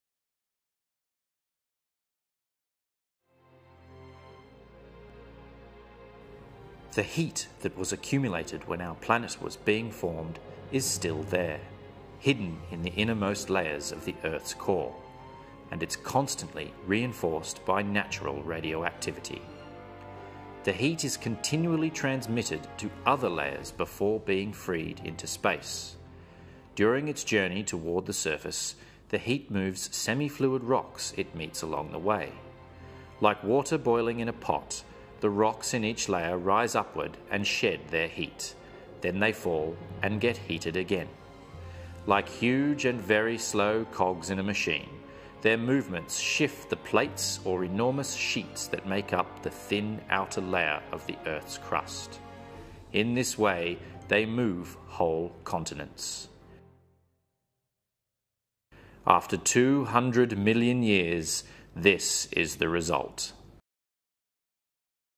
NTCES Ch1a 'Earth's Structure'
Our planet is made of many layers, like an onion. On the outside is the hard crust sitting on top of the mantle. On the inside is the solid core, which is surrounded by the liquid outer core.